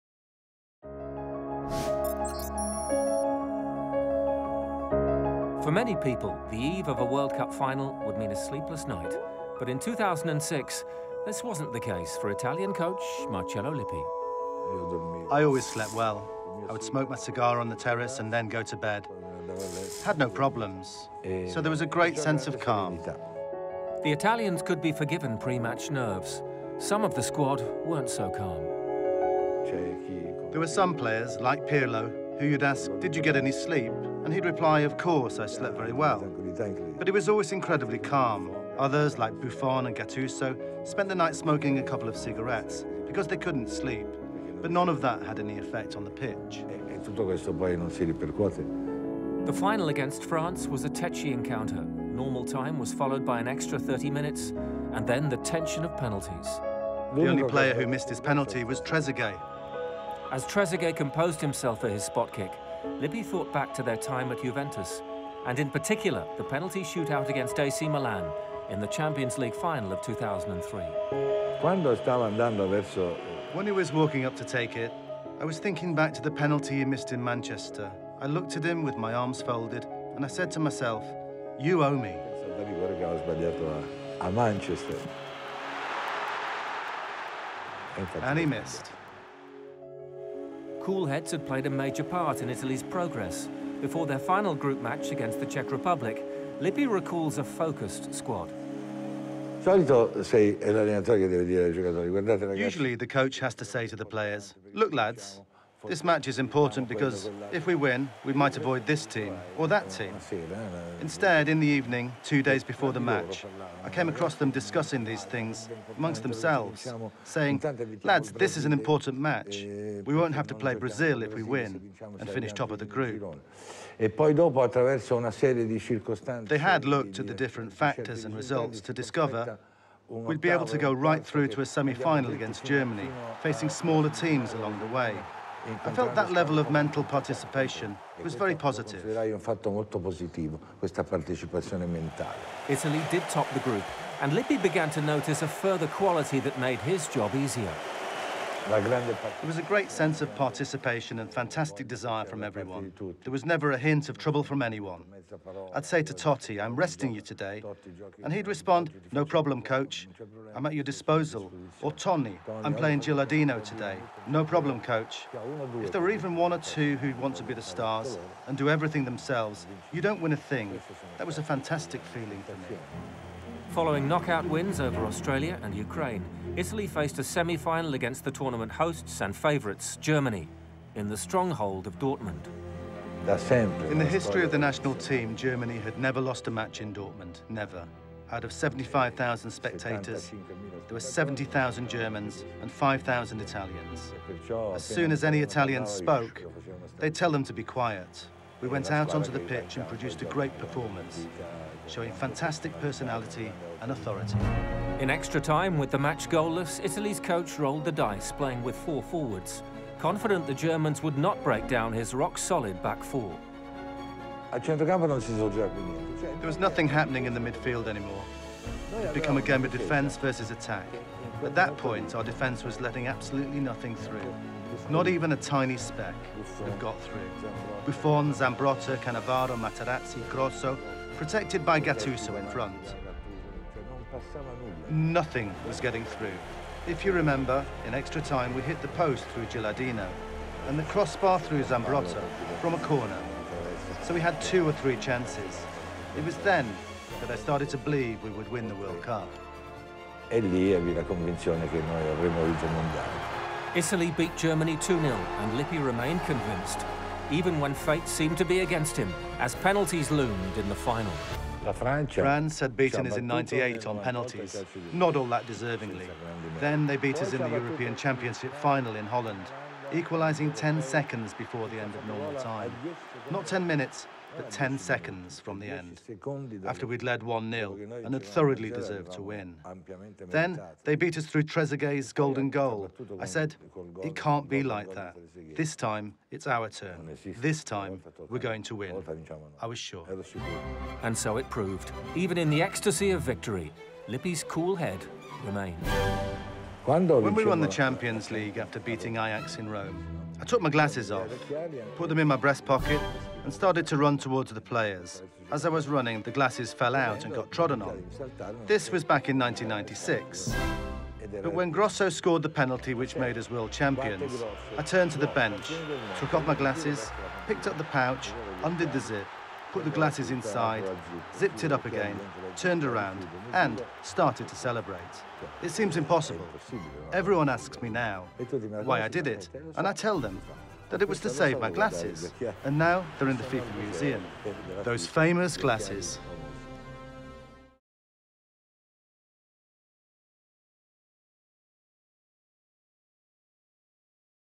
Lippi relives Italy's 2006 triumph
Italy's 2006 FIFA World Cup-winning coach gives a revealing interview with FIFA Football about the Azzurri's run in Germany.